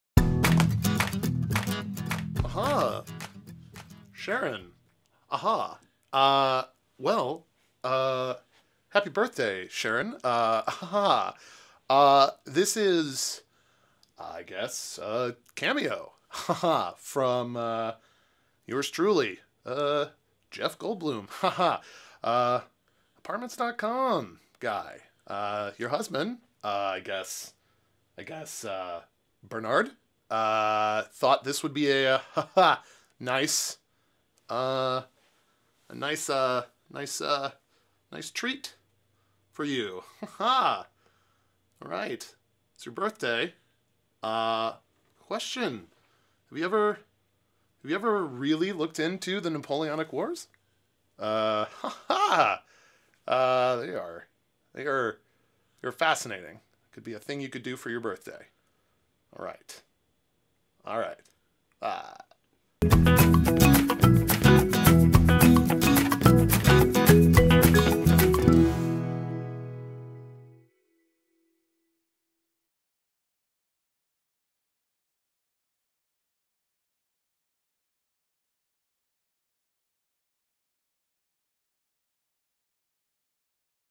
Jeff Goldblum Cameo
A special birthday message from everyone's favortie eccentric celebrity.